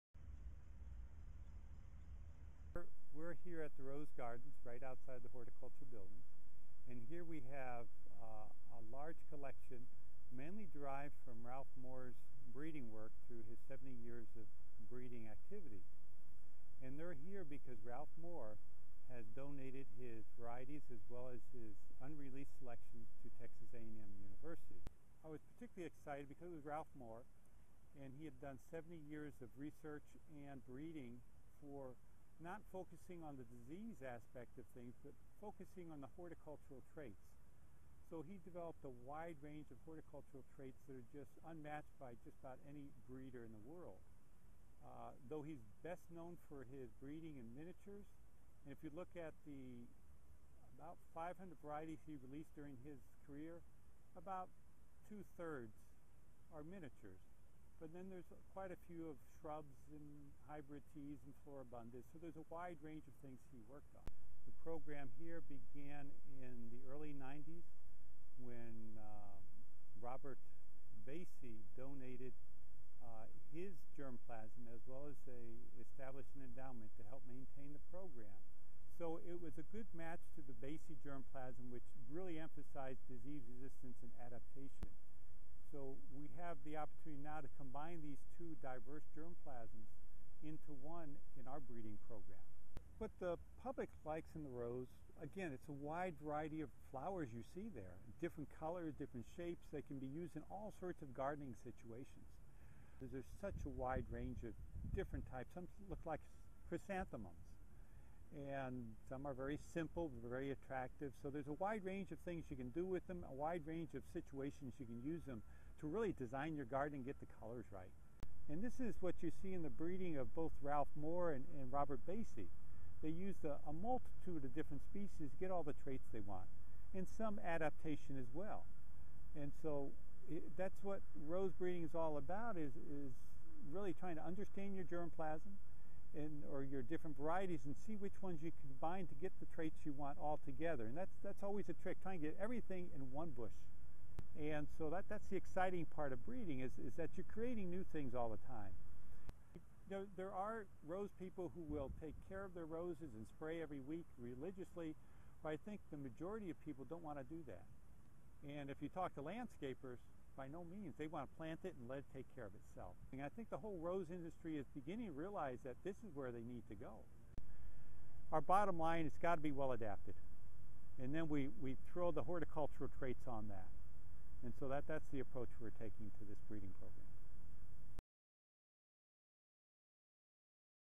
The Science of Roses
Dr. David Byrne, Texas AgriLife Research rose breeder, talks about the science of roses. He's using the horticultural traits from the Ralph Moore collection and the disease resistance and heat tolerant traits from the Dr. Robert Basye collection to breed new varieties. Both collections were donated to Byrne's work at Texas A&M.